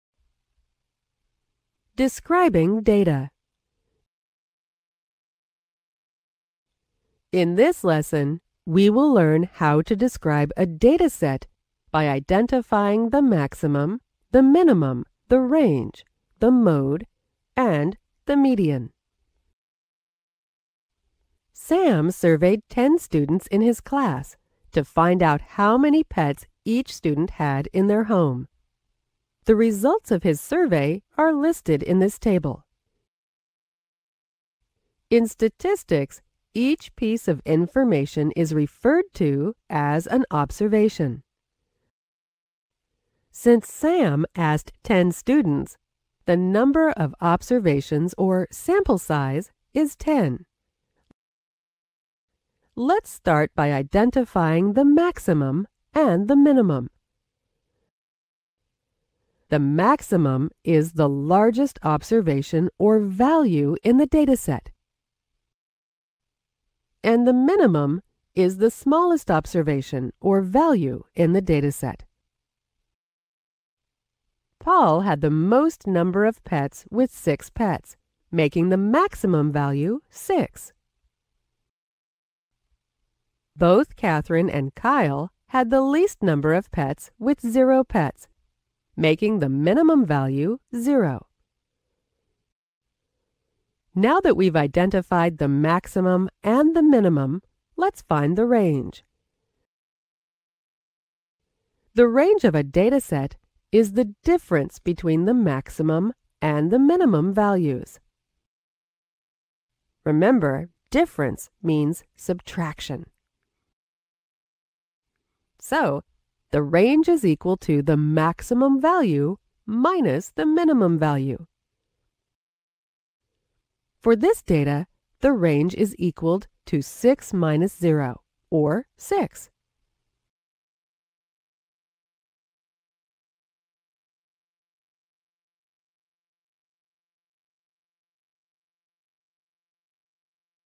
Describing Data (Part 1)
Describe a data distribution (number of observations, median, range, mode, maximum and minimum).

Now free and open to everyone.